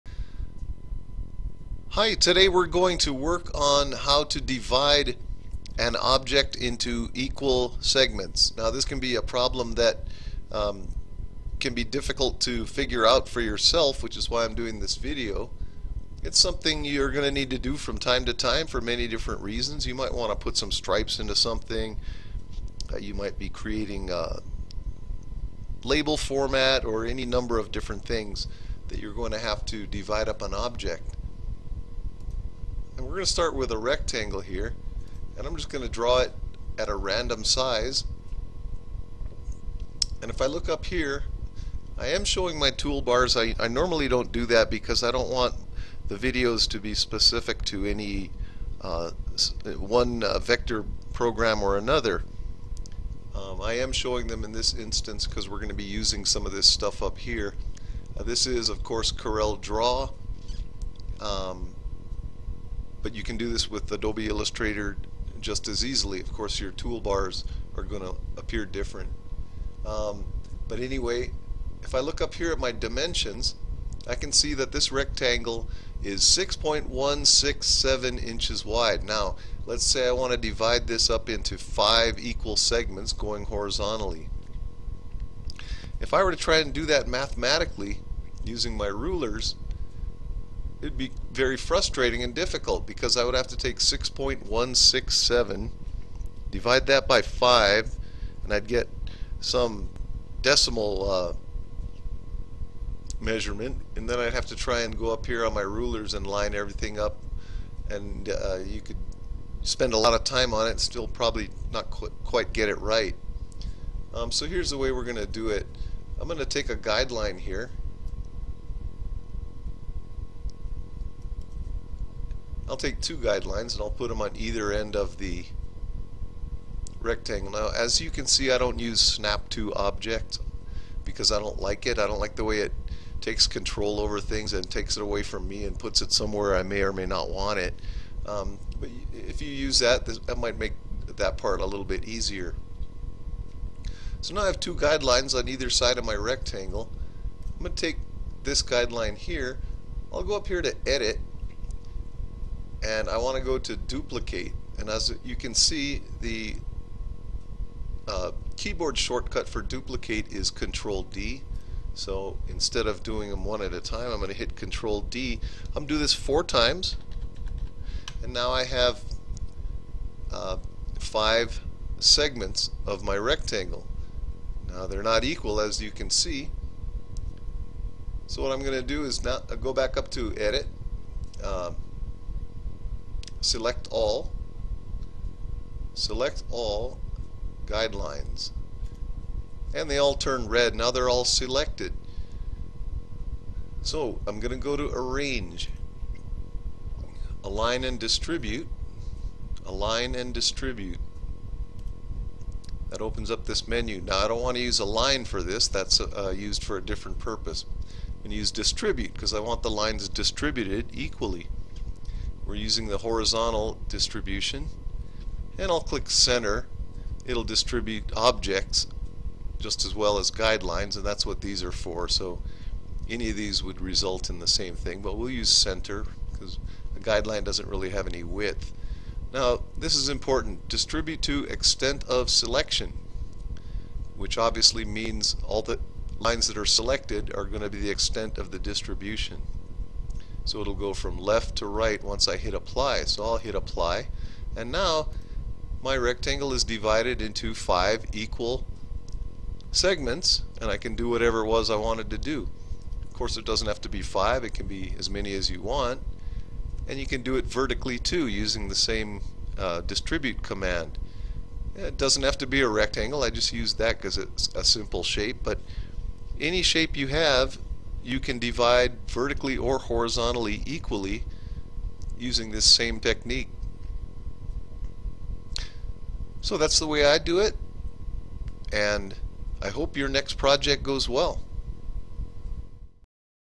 Vector drawing - Dividing an object into equal segments
How to easily divide up an object equally in Corel Draw or Illustrator. This is a basic procedure but it can be frustrating to try and figure out yourself.